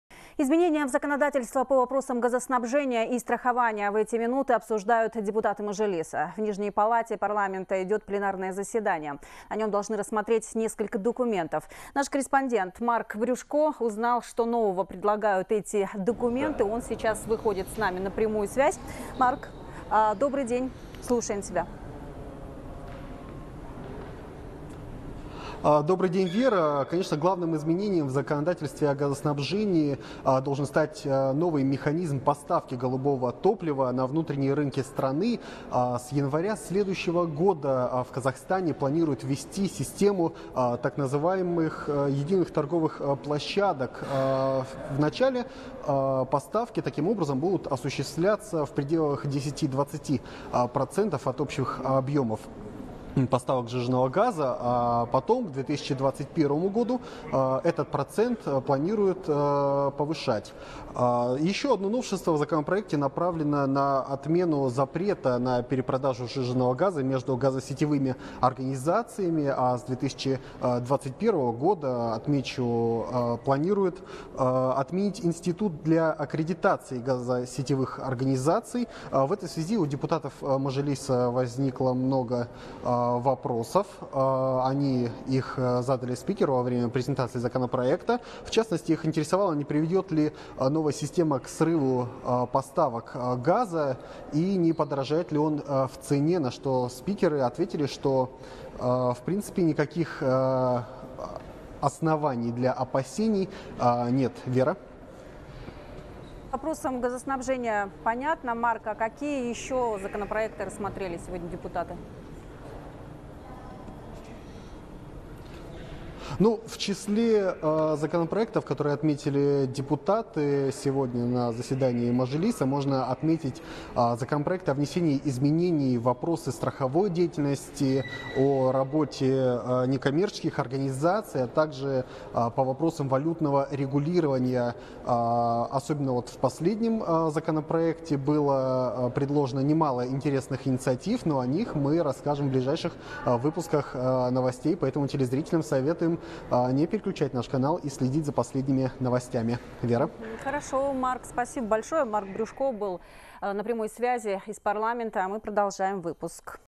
Изменения в законодательство по вопросам газоснабжения обсуждают депутаты Мажилиса
Изменения в законодательство по вопросам газоснабжения и страхования в эти минуты обсуждают депутаты Мажилиса.

В нижней палате Парламента идет пленарное заседание. На нем должны рассмотреть несколько документов.

Марк Брюшко, корреспондент:

- Главным изменением в законодательстве по вопросам газоснабжения должен стать новый механизм поставки голубого топлива на внутренние рынке страны. С января следующего года в РК планируют ввести систему единых торговых площадок. Таким образом поставки будут осуществляться в пределах 10-29% от общих объемов. Еще одно новшество направлено на отмену запрета перепродажи сжиженного газа между газосетевыми организациями. С 2021 года планируют отменить институт для аккредитации газосетевых организаций.